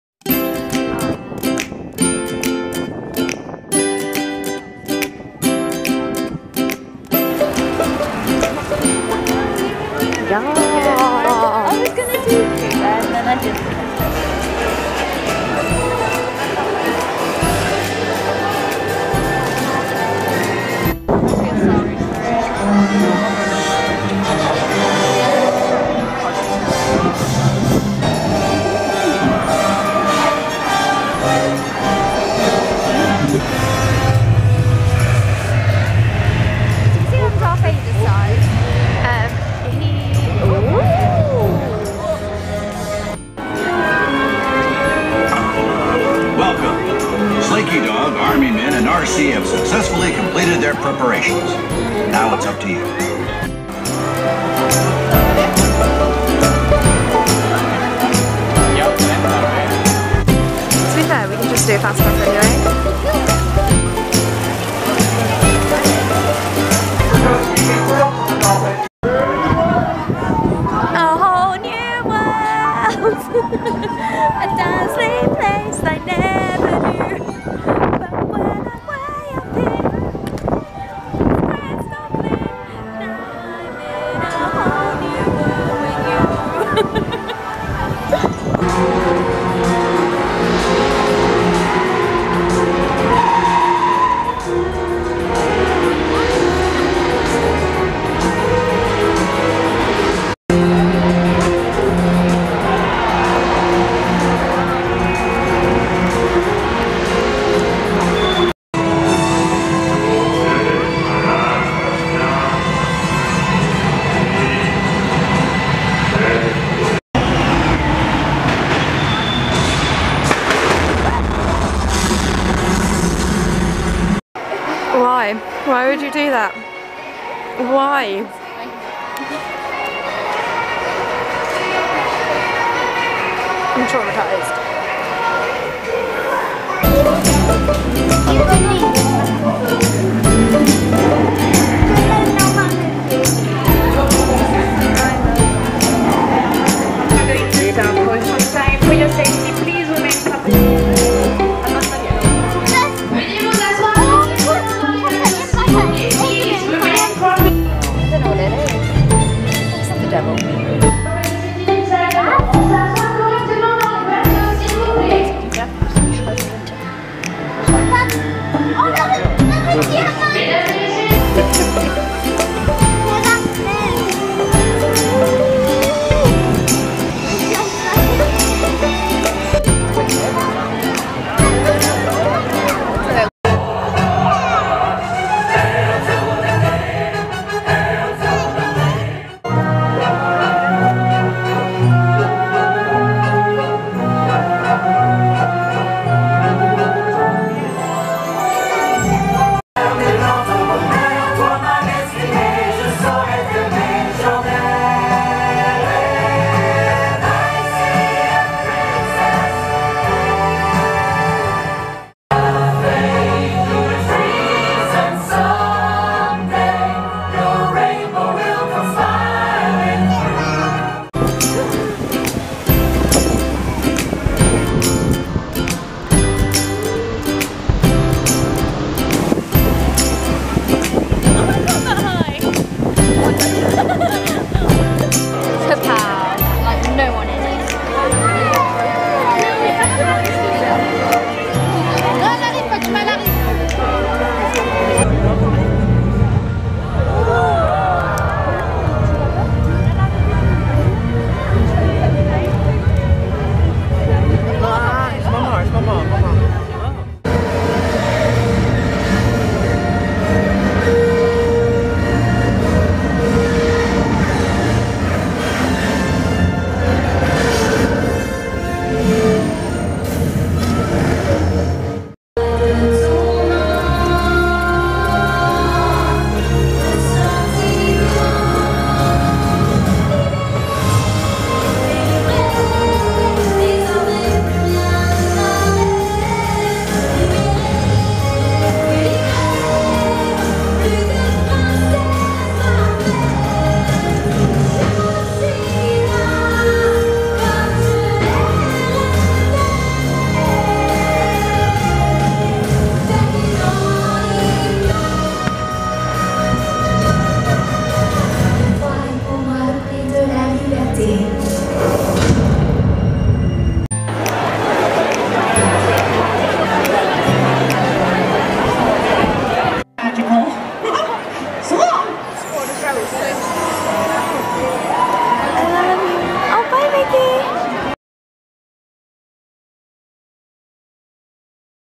Paris Adventure Pt.4- More Disneyland!
Today we explored the Disney Studios and then ventured back into the Magic Kingdom for more shows and fireworks!
Belle
xoxo